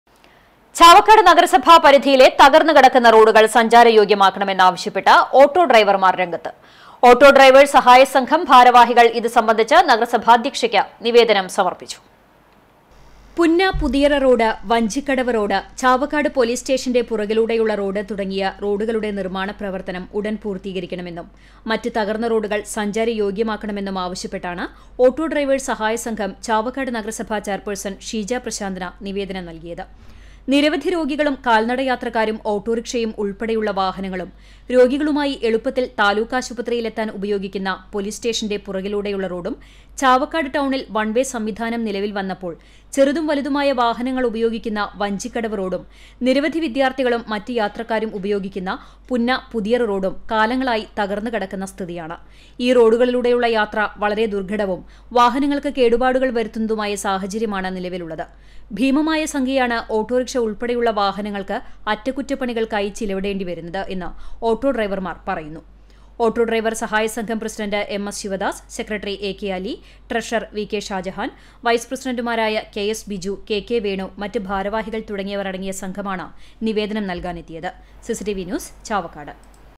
തകര്‍ന്ന് കിടക്കുന്ന റോഡുകള്‍ സഞ്ചാരയോഗ്യമാക്കണമെന്ന് ആവശ്യപ്പെട്ട് ഓട്ടോ ഡ്രൈവര്‍മാര്‍ രംഗത്ത്
ചാവക്കാട് നഗരസഭ പരിധിയിലെ തകര്‍ന്ന് കിടക്കുന്ന റോഡുകള്‍ സഞ്ചാരയോഗ്യമാക്കണമെന്ന് ആവശ്യപ്പെട്ട് ഓട്ടോ ഡ്രൈവര്‍മാര്‍ രംഗത്ത്. ഓട്ടോ ഡ്രൈവേഴ്‌സ് സഹായ സംഘം  ഭാരവാഹികള്‍ ഇത് സംബന്ധിച്ച് നഗരസഭാധ്യക്ഷയ്ക്ക് നിവേദനം സമര്‍പ്പിച്ചു.